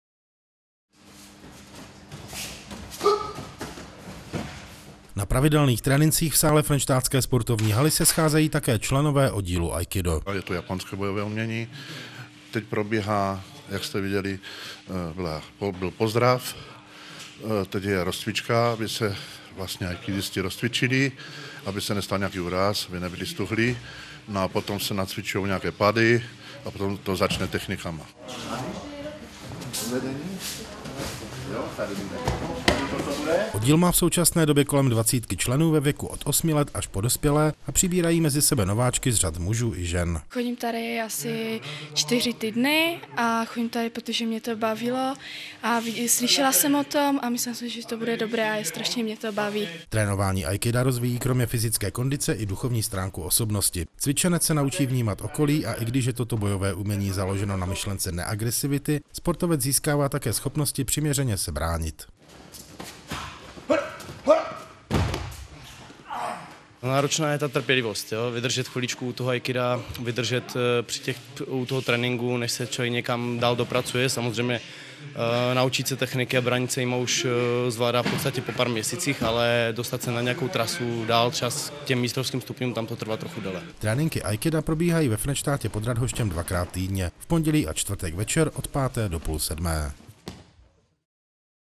Oddíl Aikida přibírá ve Frenštátě nové členy
Bojové umění, které rozvíjí psychické schopnosti, fyzičku a učí sebeobraně vhodné pro děti, muže i ženy. To je Aikido, jehož příznivci se ve Frenštátě pod Radhoštěm scházejí dvakrát týdně.